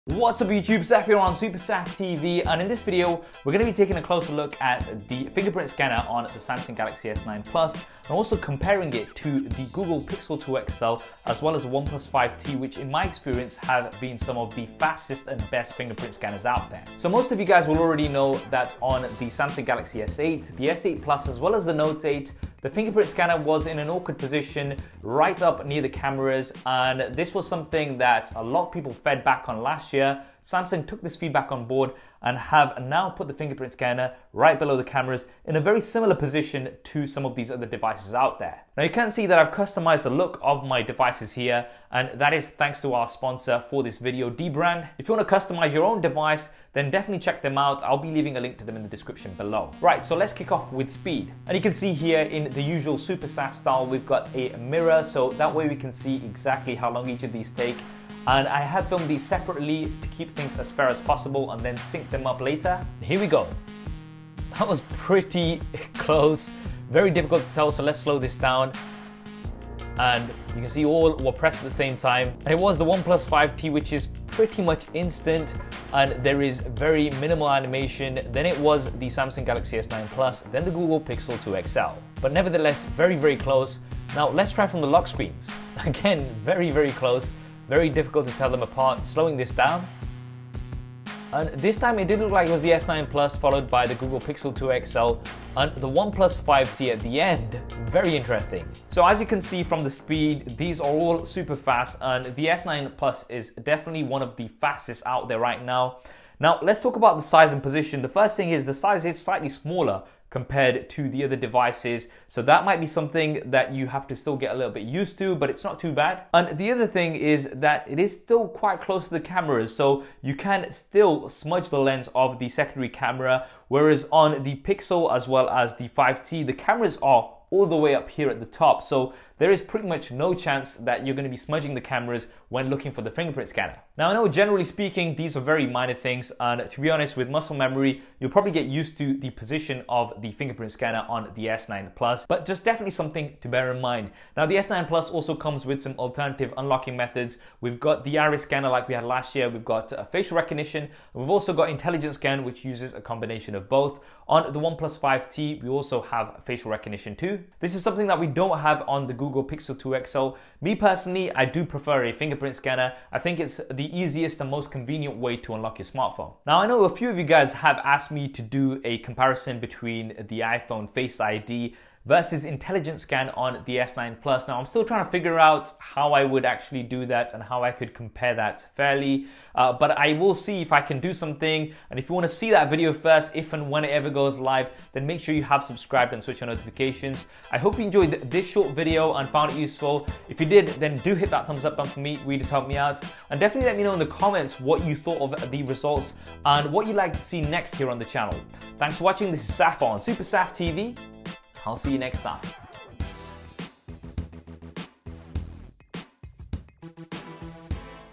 Samsung S9 vs OnePlus 5T vs Pixel 2 XL - Fingerprint Scanner Speed Test
Samsung Galaxy S9 Plus vs OnePlus 5T vs Google Pixel 2 XL Fingerprint Scanner Speed Test. Which has the fastest and best fingerprint scanner?